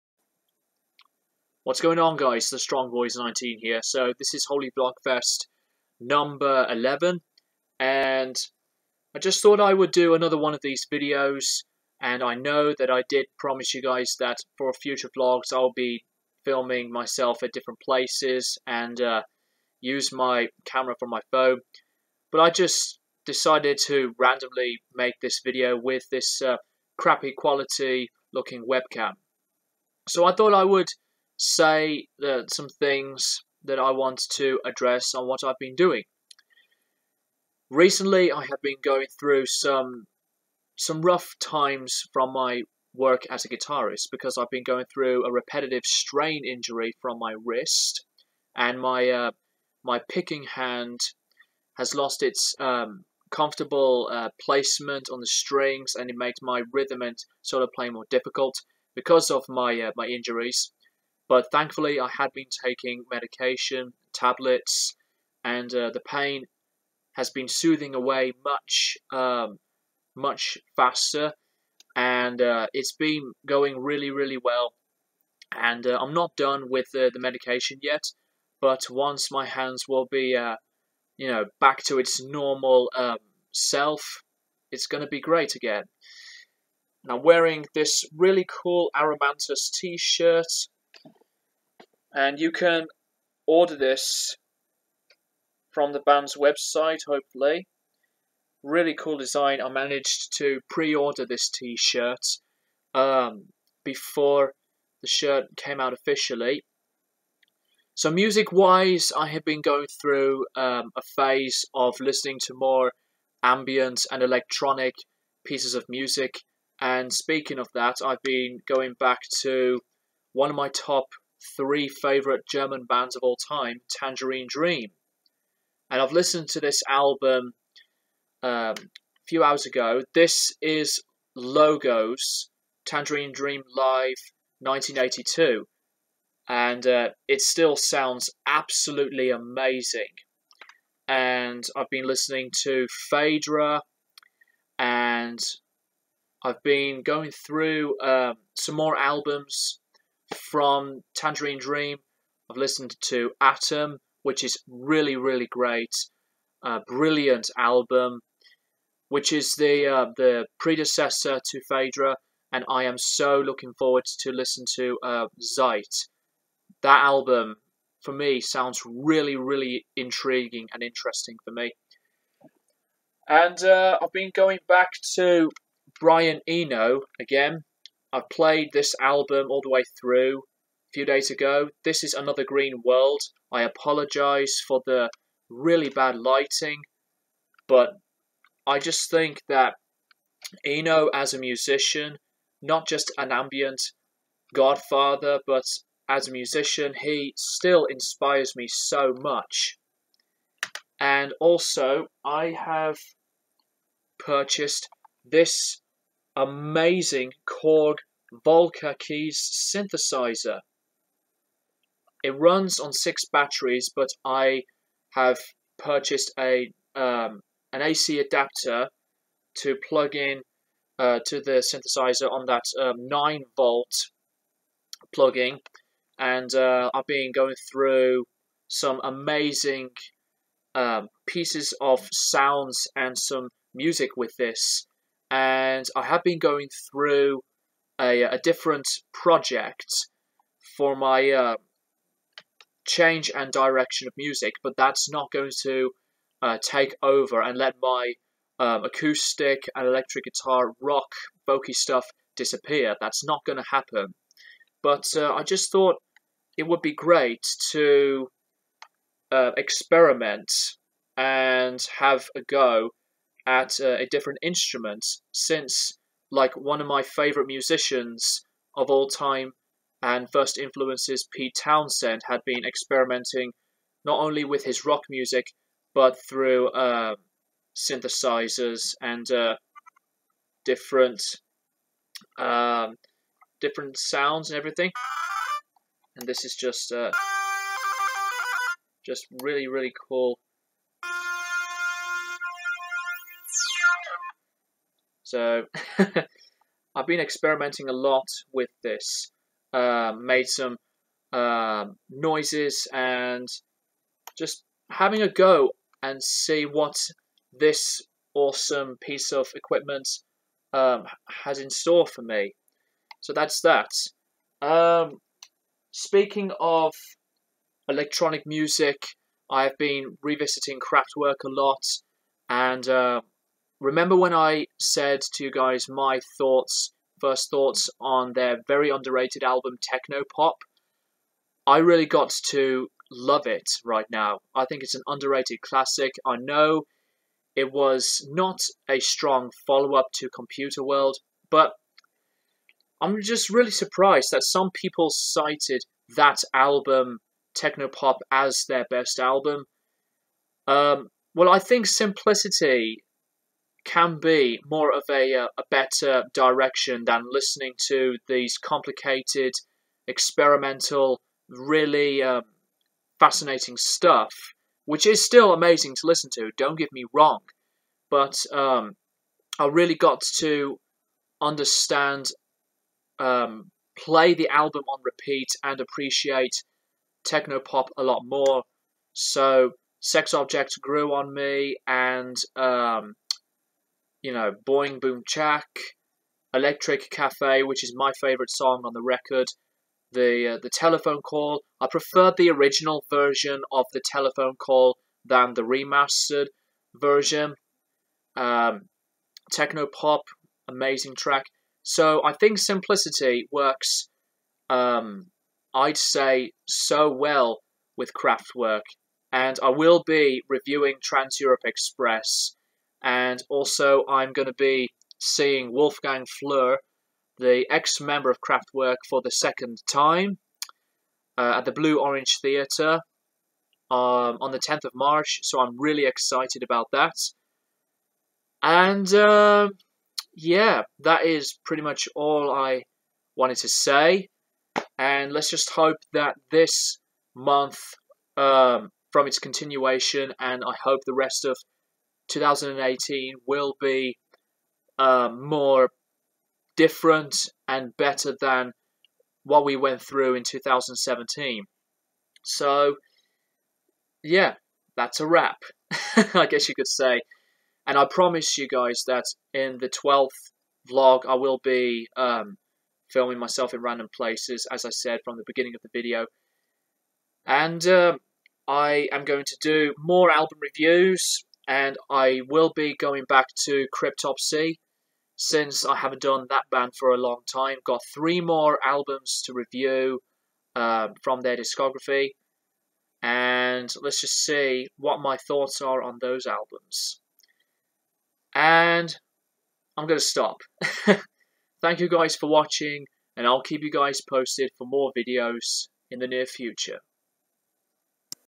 Holy Vlogfest! #11 BOUGHT A KORG SYNTHESIZER & INSPIRED FROM ENO AND TANGERINE DREAM
Here's another one for you guys and for this one I've been through a different listening experience on another one of my favourite genres